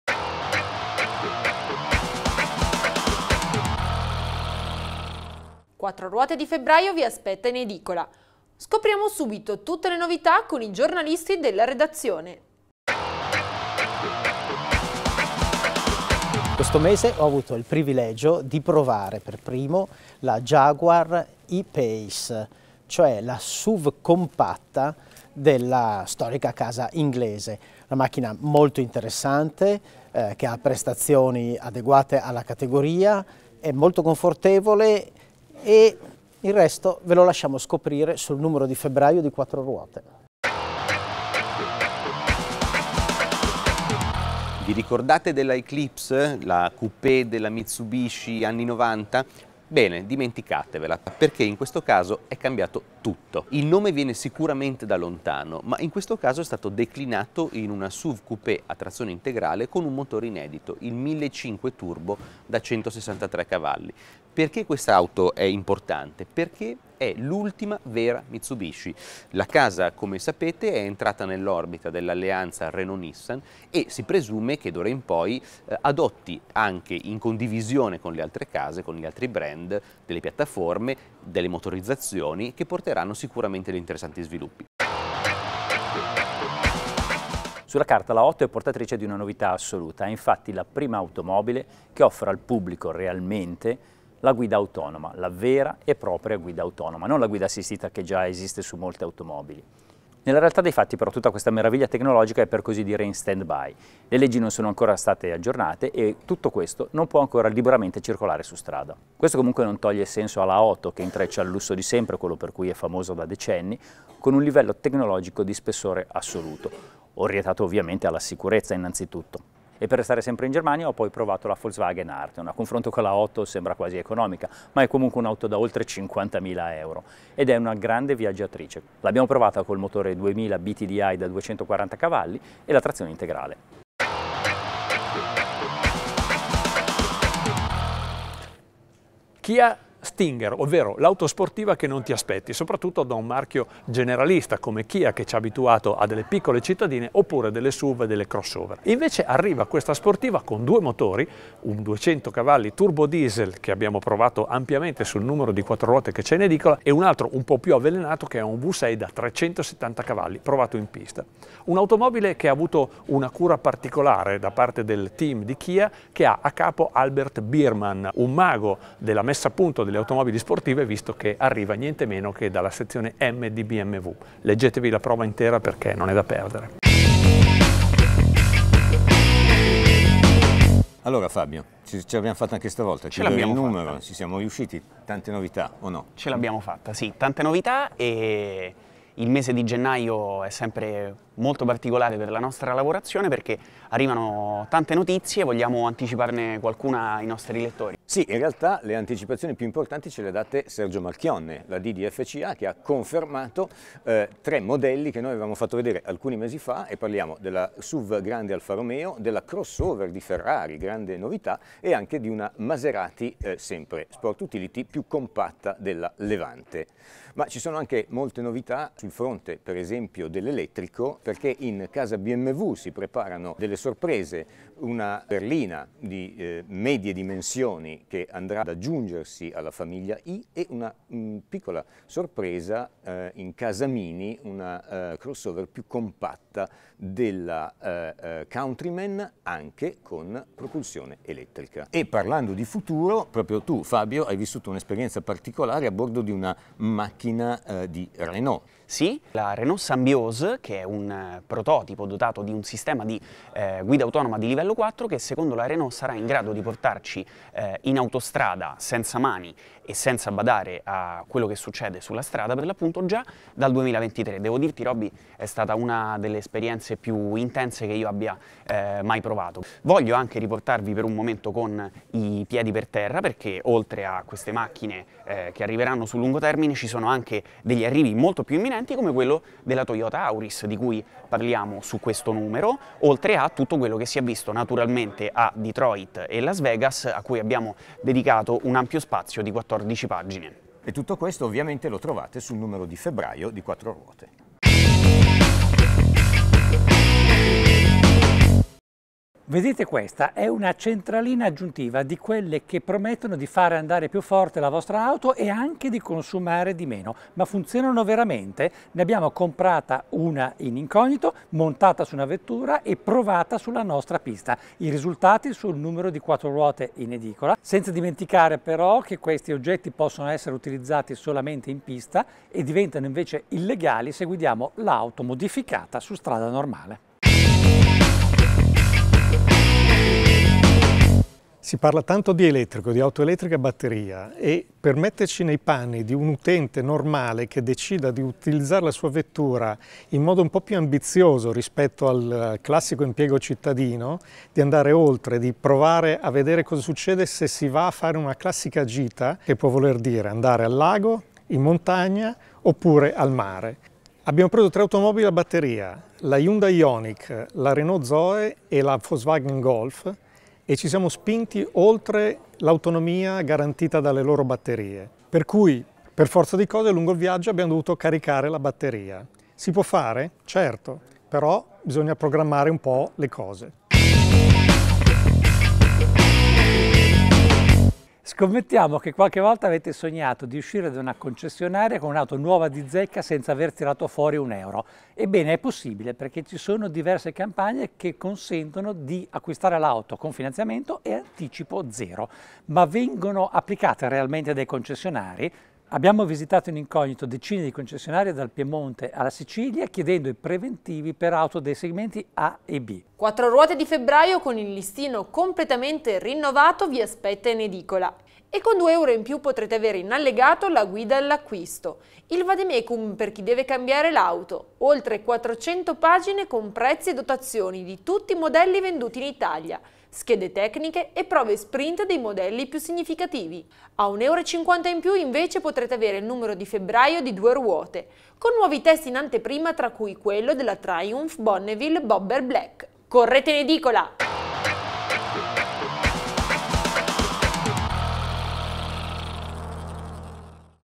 Quattroruote di febbraio vi aspetta in edicola!
Dalla prova della Jaguar E-Pace, che campeggia in copertina, alla Kia che non ti aspettavi, la Stinger, passando per l'Audi A8 e la Mitsubishi Eclipse Cross. Questo e molto altro tra inchieste, prime impressioni di guida e le novità dai Saloni di Las Vegas e Detroit nel numero di febbraio di Quattroruote, da oggi in edicola!